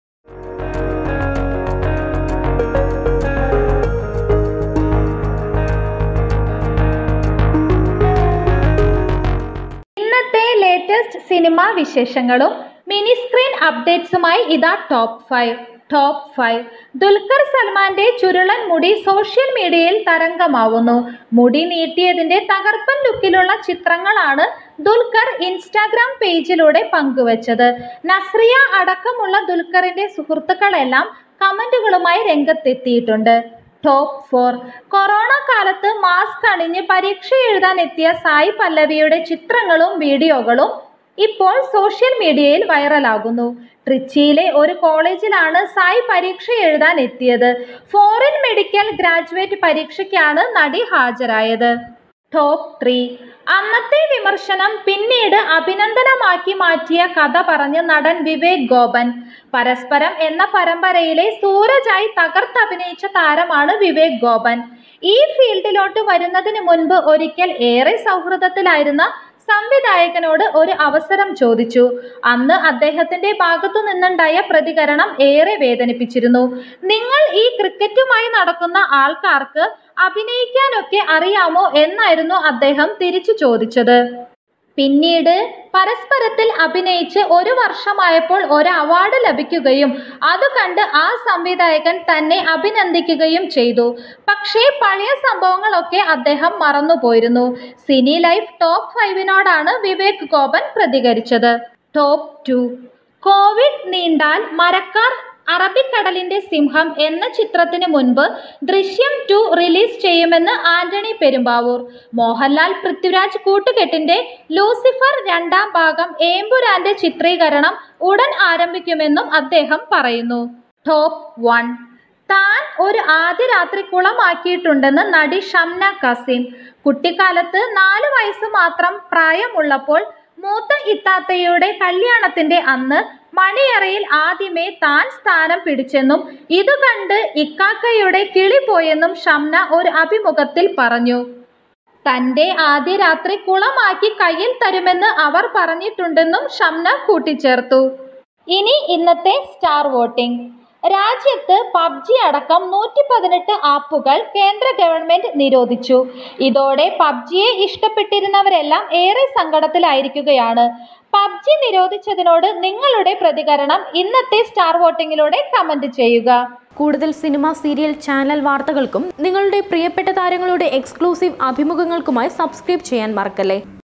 മാസ്ക് അണിഞ്ഞു പരീക്ഷയ്ക്കെത്തിയ സായ് പല്ലവി...ദൃശ്യം 2 ആദ്യമെത്തും.. l Top 5
ഒടുവിൽ പബ്‌ജിയും നിരോധിച്ചു... നിരാശയിൽ പബ്‌ജി പ്രേമികൾ. ഞാൻ ഒരു ആദ്യരാത്രി കുളമാക്കിയിട്ടുണ്ട്, എന്റെ ആദ്യരാത്രി അവരും കുളമാക്കും-തുറന്നുപറഞ്ഞ് ഷംന കാസിം... പരസ്പരത്തിലെ സൂരജ് പറയുന്ന ആ പഴയ കഥ...പുത്തൻ വാർത്തകളുമായി ടോപ് 5